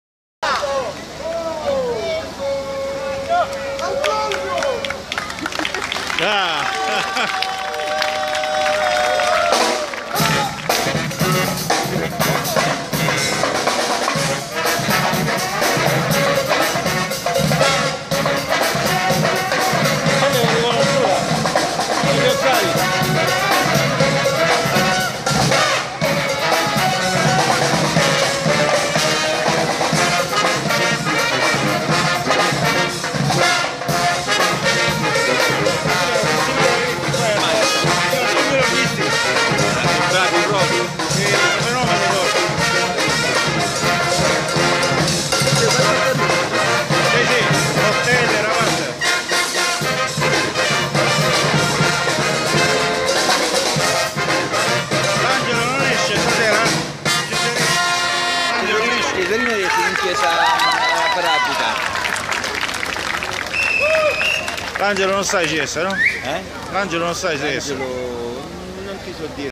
Salento orchestra a collepasso
La salento orchestra e venuta nel mio piccolo paese di Collepasso amici spero vi piaccia